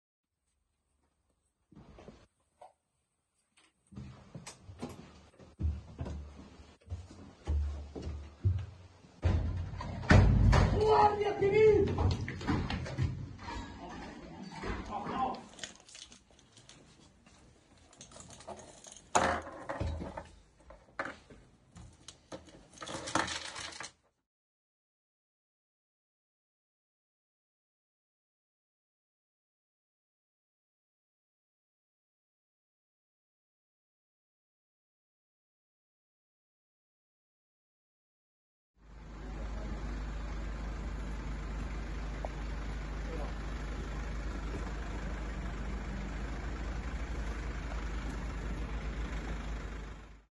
Operación 'Hábitat Ángeles' Guardia por robos en viviendas en la Comunidad de Madrid
La Guardia Civil ha detenido a dos personas de origen albanés que seleccionaban viviendas unifamiliares y perpetraban los robos mientras los moradores estaban en las plantas bajas.

La operación 'Hábitat Ángeles' desarrollada por el Grupo de Delincuencia Organizada de la Unidad Orgánica de Policía Judicial de la Comandancia de Madrid, ha logrado detener a los integrantes de un grupo criminal especializado en la comisión de robos con fuerza en una treintena de viviendas situadas en media docena de localidades de la Comunidad de Madrid, como Boadilla del Monte, Las Rozas, Pozuelo de Alarcón, entre otras.